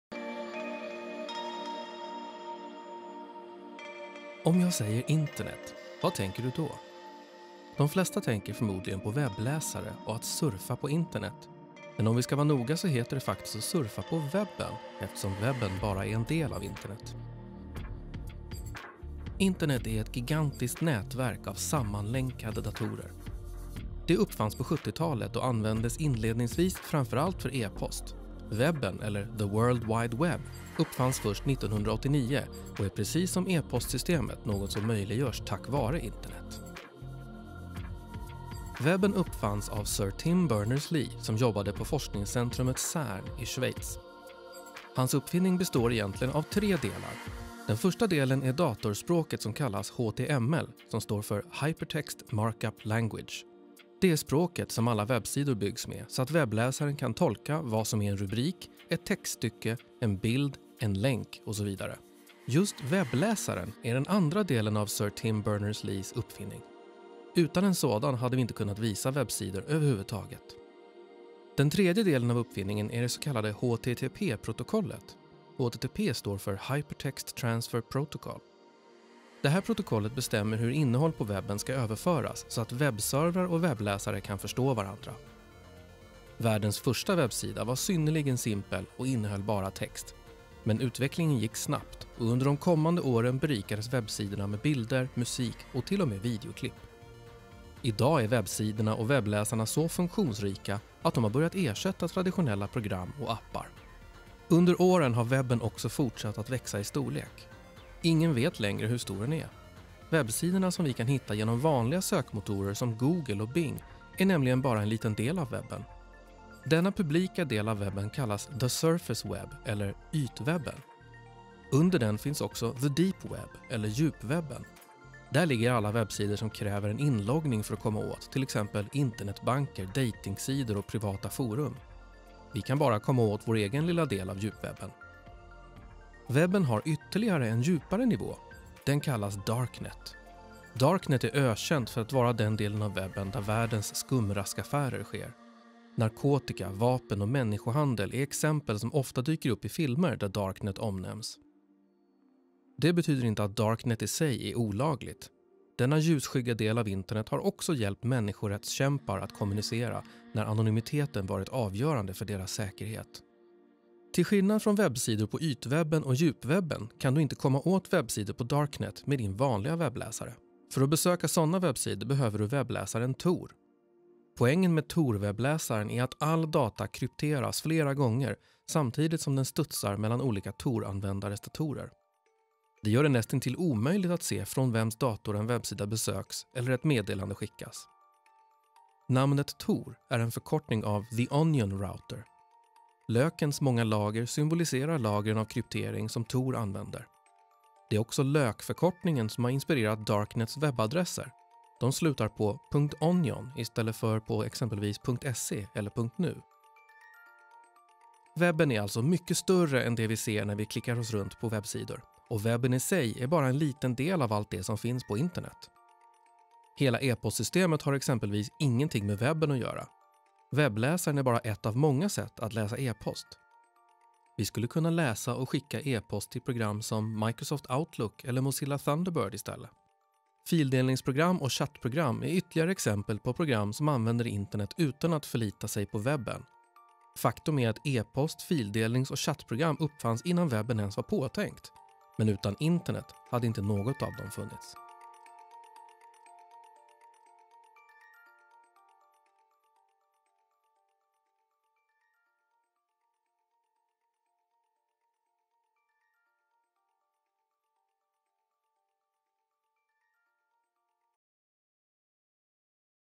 Enkelt förklarat: Är internet samma sak som webben? | Internetkunskap
Webben är en fantastisk uppfinning som har gett oss människor helt nya möjligheter. Men att surfa på webben är bara en del av allt som går att göra med internet. Den webb du surfar runt på är dessutom bara en liten del av hela webben. Djupwebben och darknet finns också på webben – även om du inte kommer åt dem med enkla länkklick. Så här funkar webben!

I Sverige använder de flesta internet varje dag, och vi tar för givet att det alltid ska funka. Men hur funkar tekniken bakom och vem är det som bestämmer över internet? Lär dig mer om vad som gör att du hittar rätt webbsida när du surfar, vilka lagar som gäller och vilka som styr över nätet.